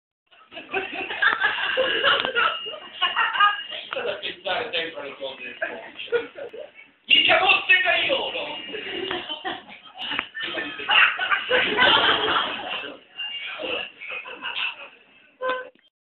Quanto mi dai se ti uccido 1
Dall'esilarante spettacolo "quanto mi dai se ti uccido?" di Ippolito Chiarello, presentato al Caffè Letterario di Lecce  il 24 marzo 2010